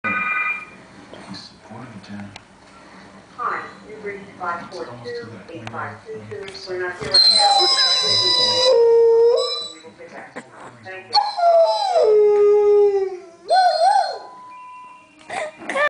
My dog howling at the voicemail
This is my dog howling at the voicemail
Not the best quality I know, but this was the best I could do on short notice.